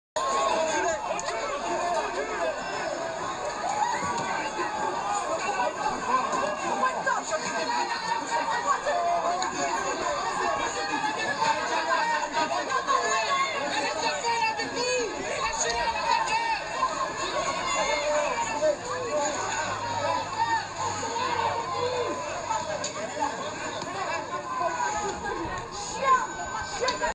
5 policiers frappent une femme
Les forces d'ordre perdent le contrôle de leurs nerfs et brutalisent une jeune femme.
Scène surréaliste à Paris le samedi 12/02/2022 durant le convoi de la liberté.